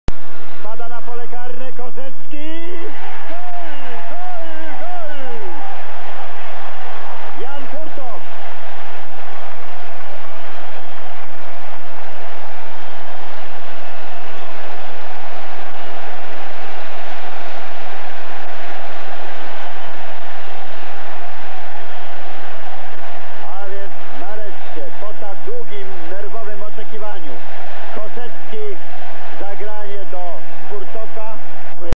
Jan Furtok - gol strzelony ręką z San Marino (1993.04.28)
Mecz Polska - San Marino w ramach eliminacji Mistrzostw Świata 1994.

Jan Furtok strzela jedyną bramkę w meczu, bramkę strzeloną ręką, ostatnią w swojej karierze w reprezentacji Polski (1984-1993, bilans 36-10).